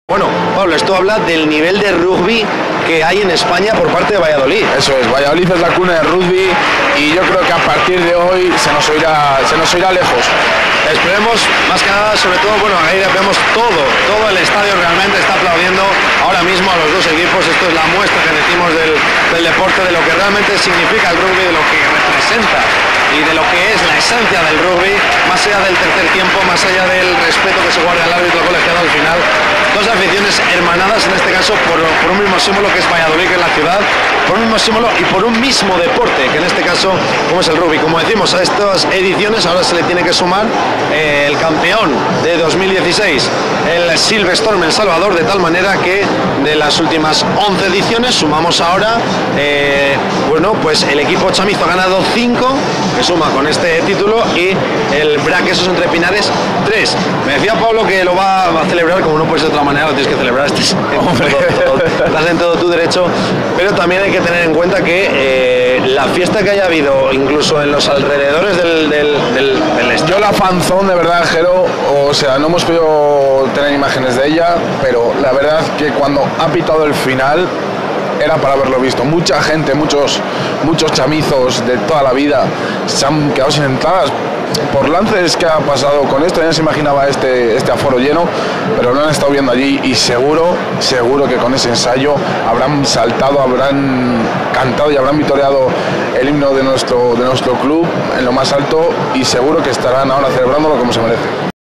Valladolid, cuna del Rugby. Final Copa del Rey rugby 2016
Con un lleno histórico del Estadio José Zorrilla, 26.000 espectadores pudieron disfrutar de la final de rugby de la Copa del Rey de Rugby 2016 entre los dos equipos vallisoletanos. Record histórico!!! Gran afición en Pucela. Narración de Jero Díez, comentarios Pablo Gonzálo Burgueño. Imagen de Javier Luis